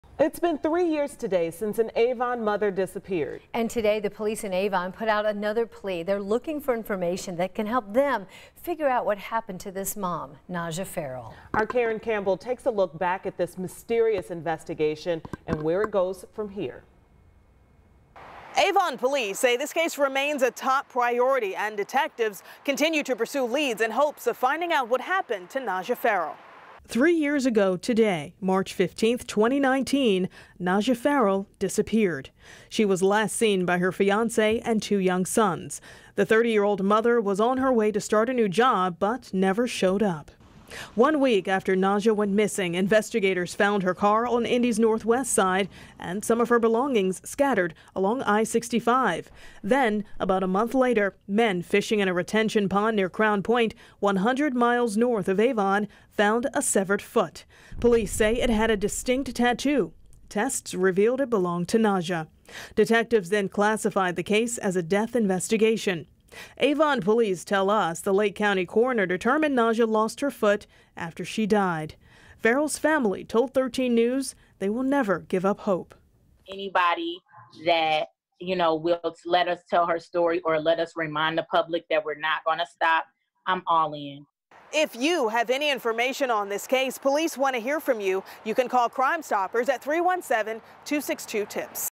What Happened To Najah Ferrell? | Three Years Later
It's been three years today since an Avon mother disappeared. Our Karen Campbell takes a look back at this mysterious investigation.